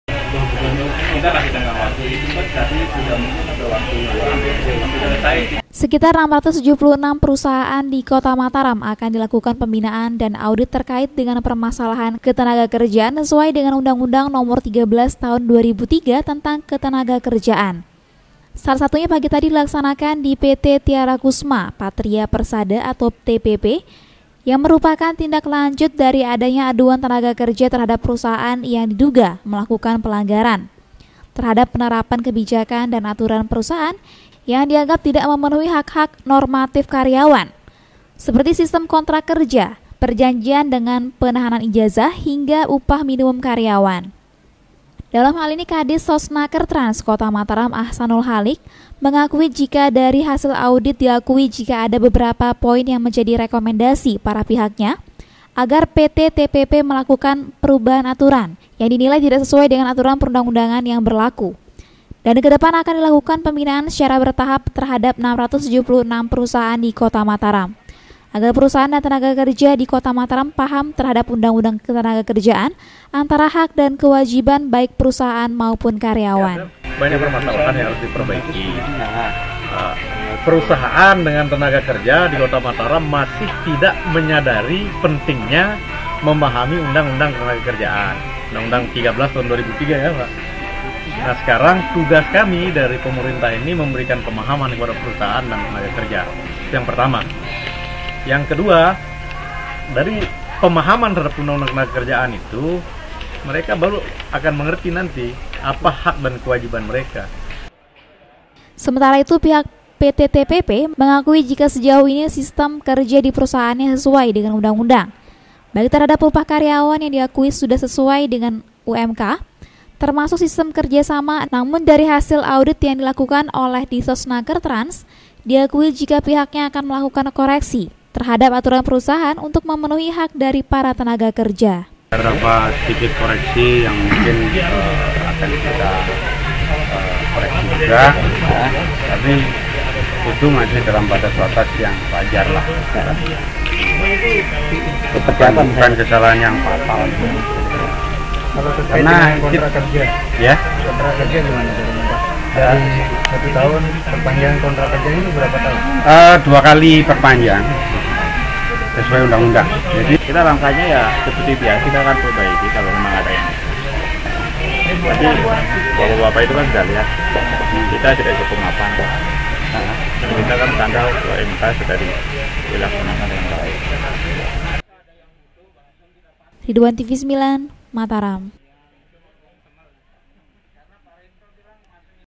676 PERSUSAHAAN AKAN DILAKUKAN PEMBINAAN
MENCUATNYA PERMASALHAN KETENAGA KERJAAN DI KOTA MATARAM AKHIR AKHIR INI, DINAS SOSISAL TENAGA KERJA DAN TRANSMIGRASI KOTA MATARAM MELAKUKAN PEMBINAAN DAN AUDIR BAGI PERUSAHAAN TERKAIT DENGAN PERMASALAHAN KETENAGAKERJAAN YANG MENJADI SALAH SATU PROSEDUR DALAM KELAYAKAN OPERASIONAL PERUSAHAAN.